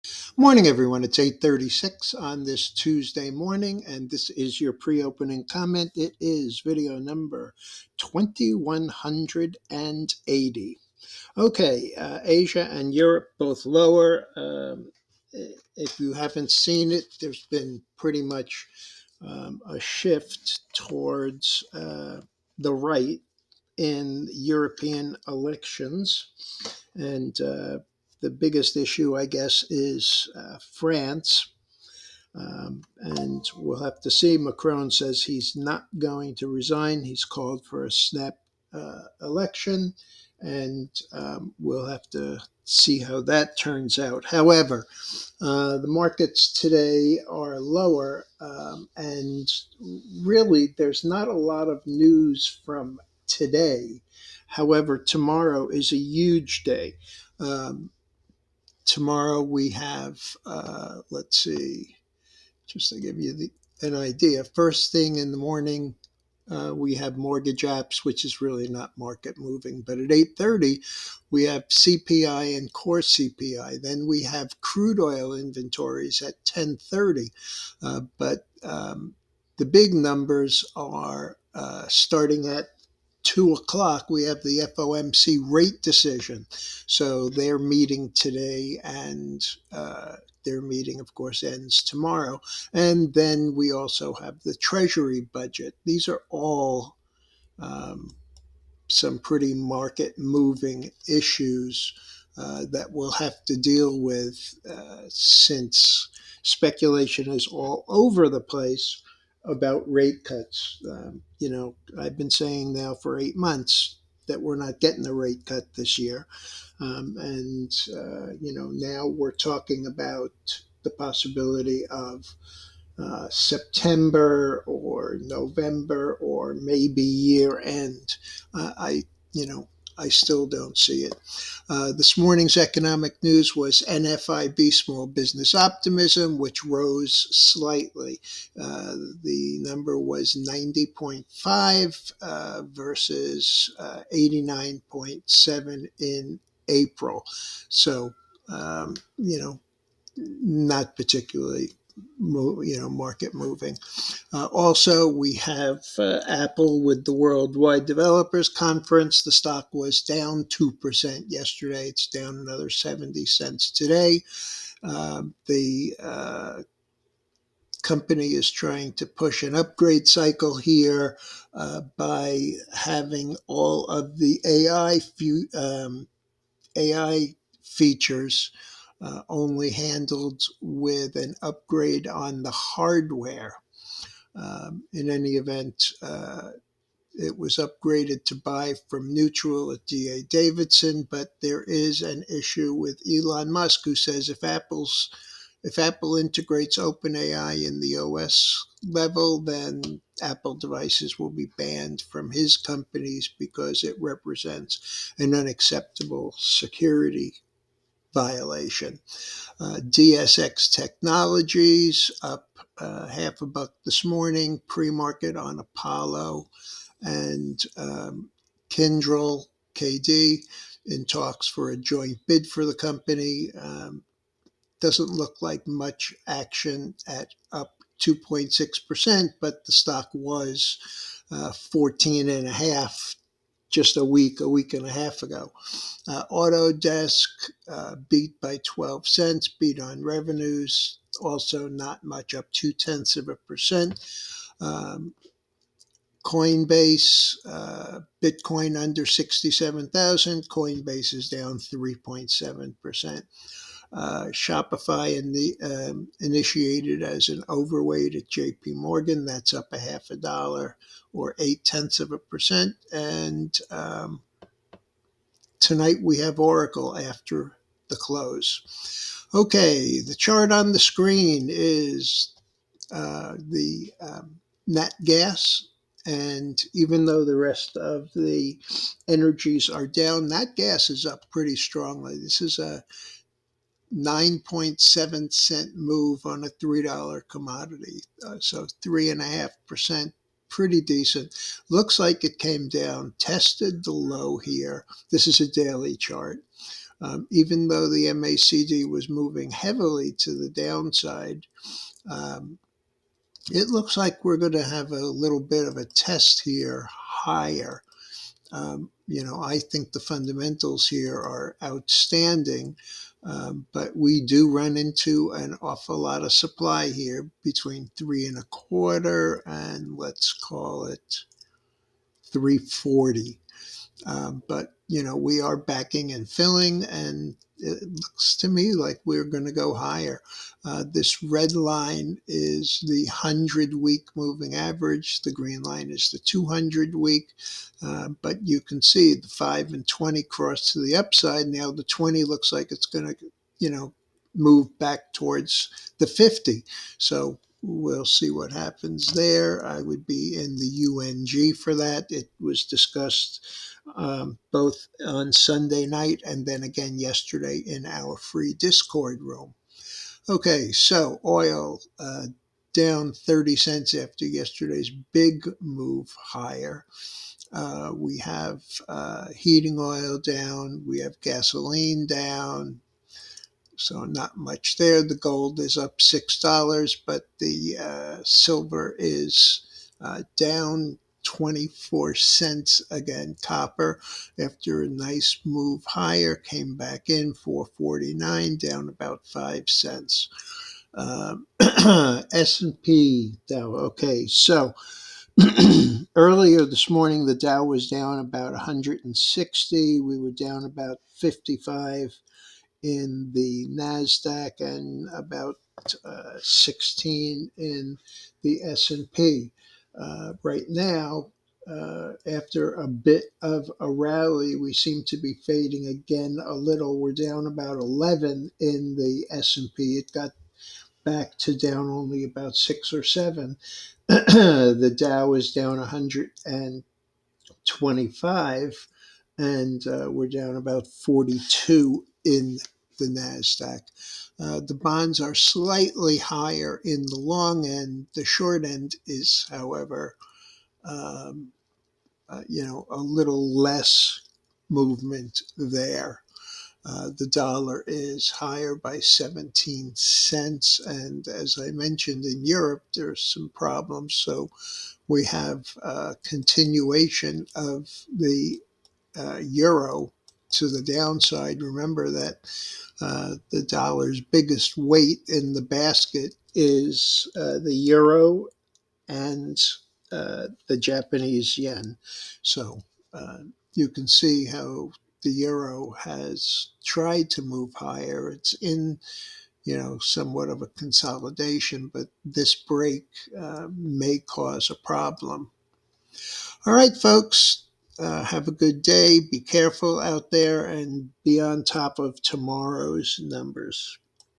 STOCKS SLIGHTLY LOWER, QUIET NEWS DAY, MARKETS AWAIT TOMORROW'S CPI, CORE, FOMC DECISION !!
PRE-OPEN  COMMENT
VIDEO #2180